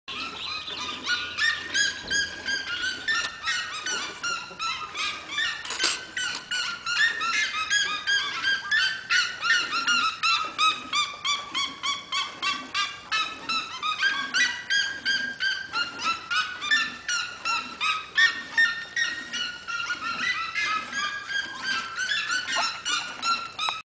SIRIEMA.MPG
Siriema no curral da casa Do Cláudio Nunes